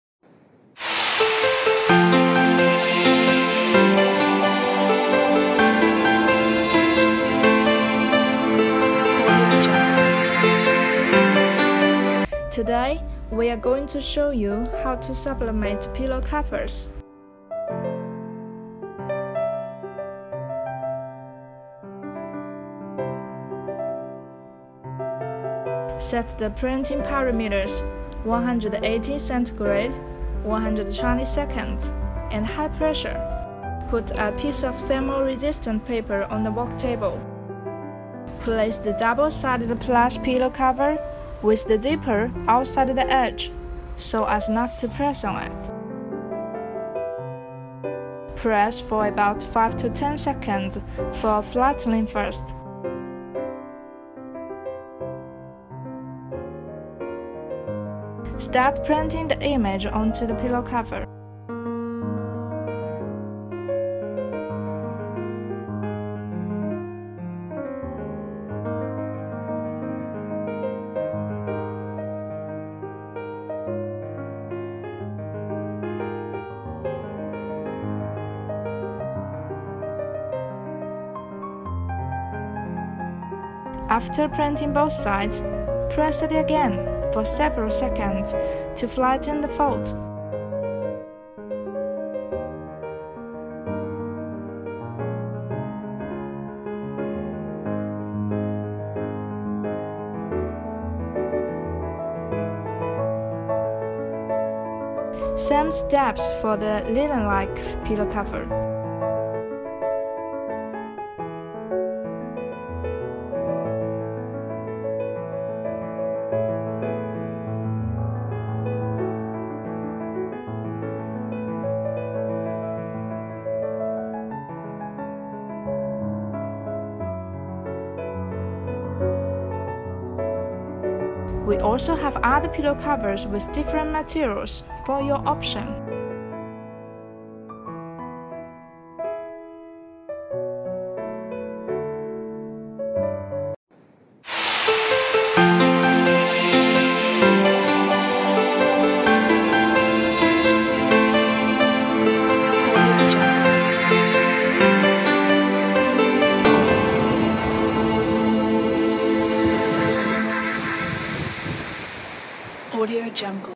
How to Sublimate Pillow Covers
BestSub’s new sublimation pillow cover made of linen is a great way to add some style to any décor. Throw our linen pillow cover imprinted with a beautiful image on your sofa; it is soft enough for you to lean against comfortably while watching TV. You can also throw it on your garden chair while enjoying happy tea time with your family and friends.